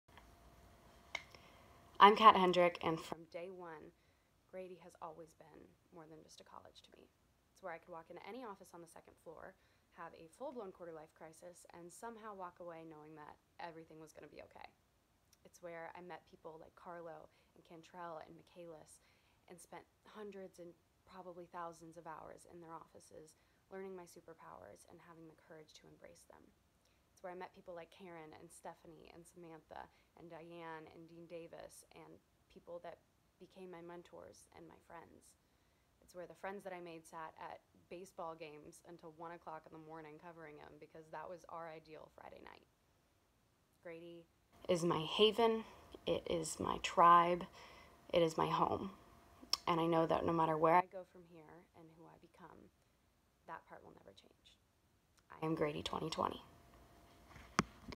Grady2020 - Cat Hendrick
Cat Hendrick reflects on her time at Grady College as part of the class of 2020.